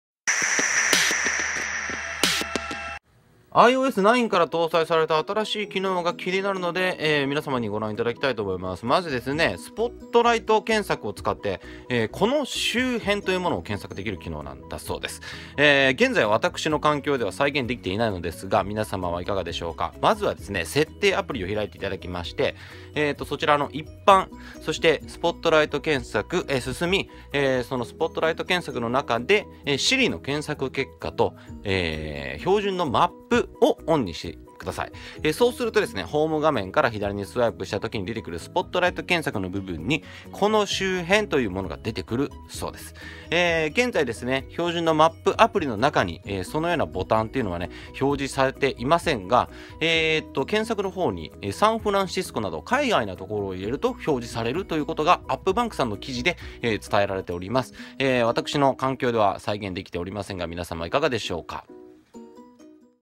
【iOS 9】近くのスポットをspotlight検索で探せる？？：1分ニュース動画
iOS9から追加されたと言われている近くのスポットを検索できるという機能を調べてみた。

引用元：【iOS 9】ホーム画面から近くの店をカンタン検索